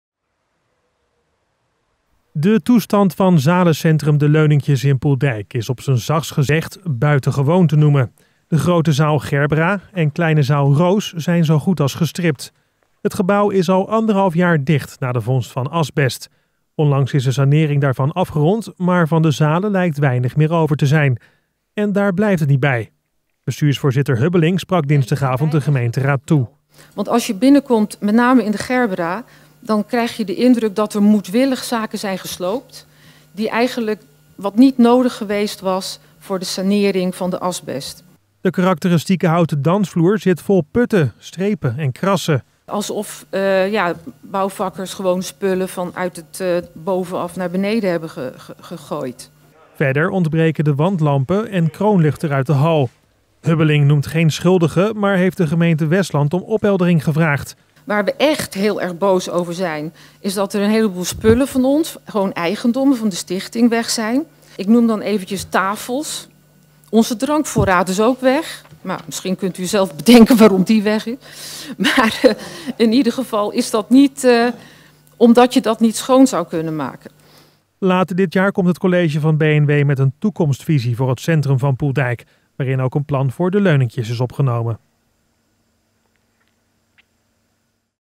Spullen 'verdwenen' uit De Leuningjes
Onbekenden hebben flink huisgehouden tijdens de sanering van zalencentrum De Leuningjes in Poeldijk. Dat zei bestuursvoorzitter Jeanet Hubbeling gisteravond, als inspreker in de Westlandse gemeenteraadsvergadering.